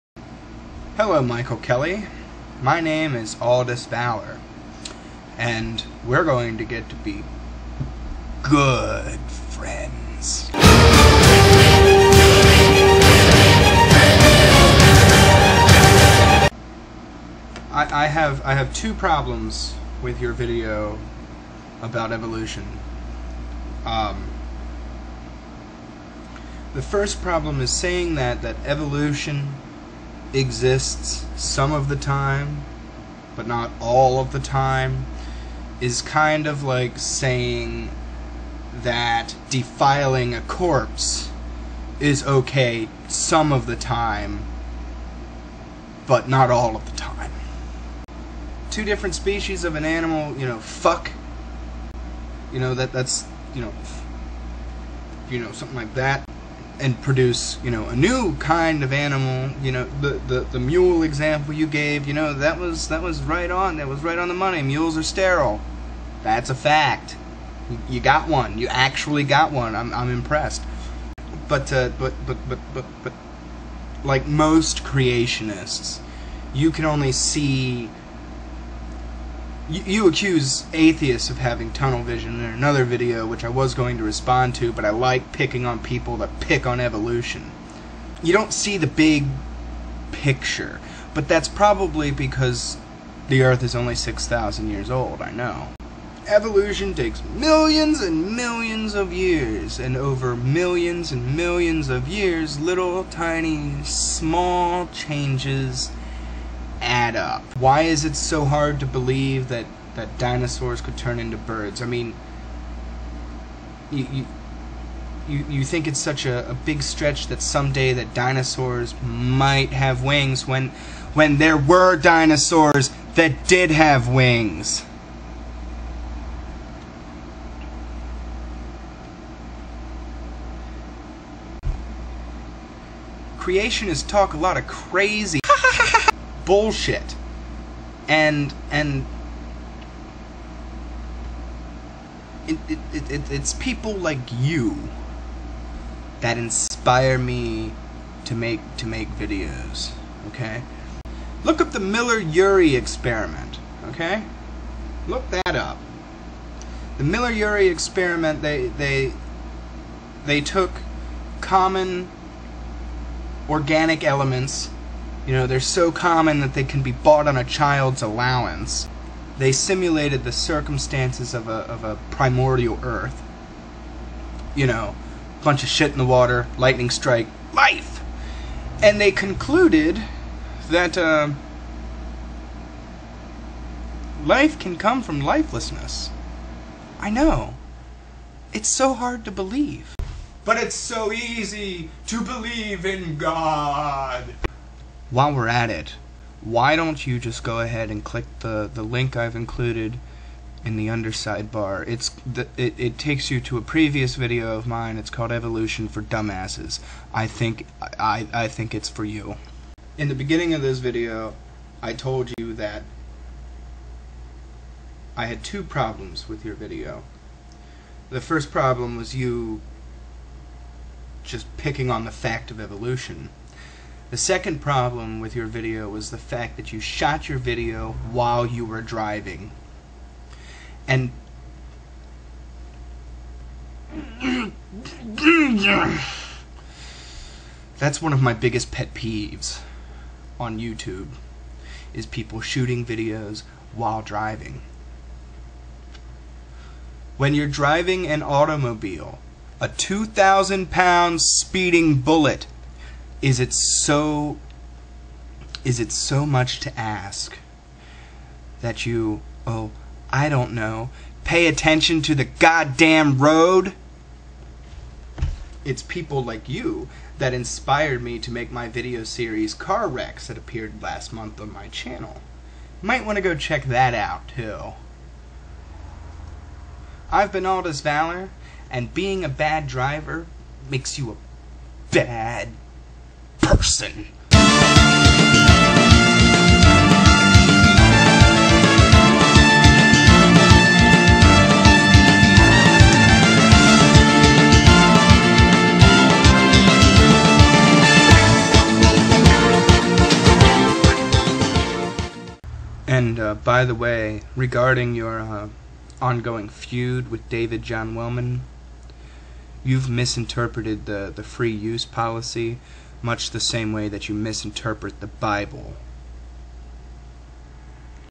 Poor Genetic Material
This video was recorded before he dropped the DMCA against djw, but wasn't saved or uploaded until after he withdrew the claim and removed the video I'm responding too. Sorry about that, but he's the one who should be apologizing. The video I was responding to was called "Micro- and Macro-Evolution," or something like that.